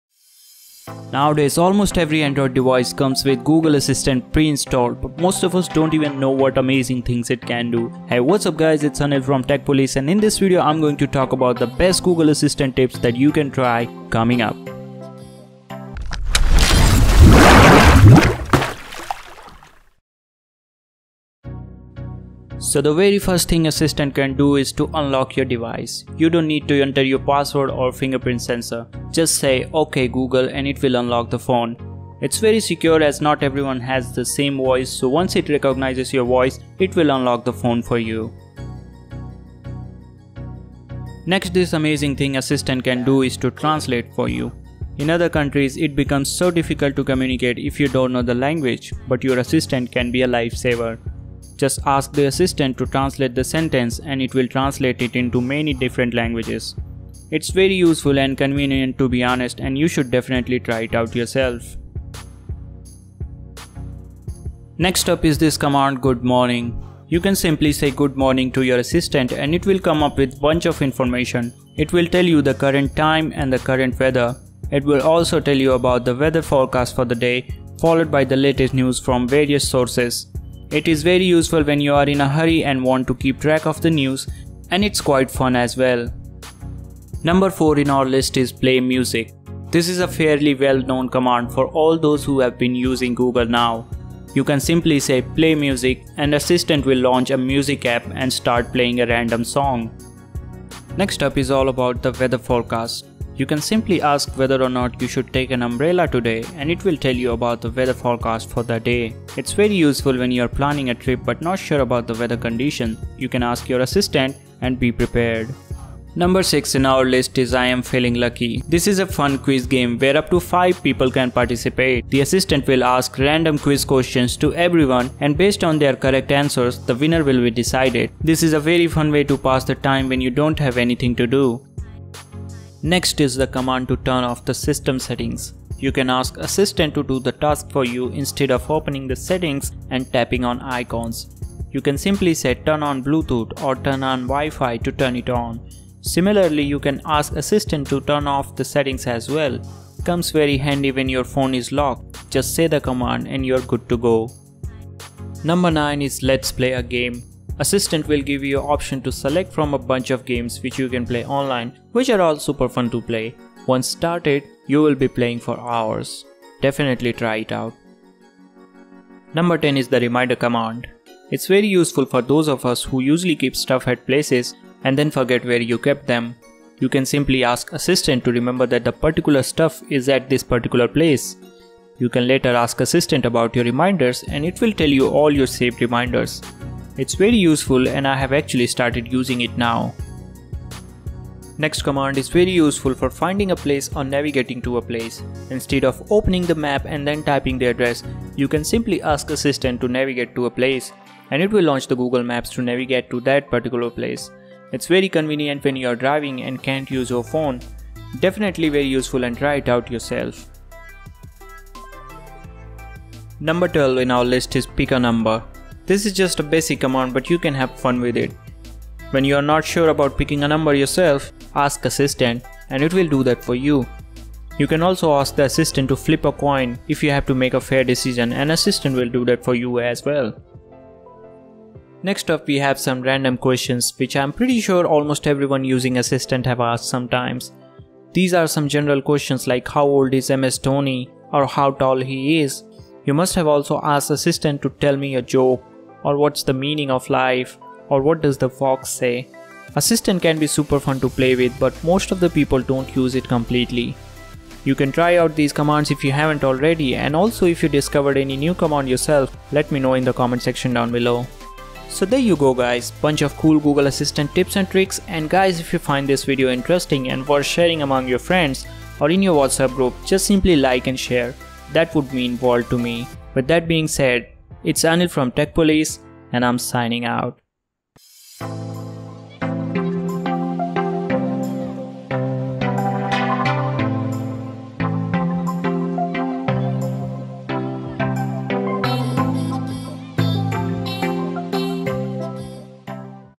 Google Assistant tricks You NEED to try
The Google Assistant from the Google Pixel devices has made its way to all Android devices running Android 6.0 Marshmallow and higher. So, if you have just got the Google Assistant on your device, we show you the best Google Assistant tricks, hidden features and Ok Google commands that will make your day that bit better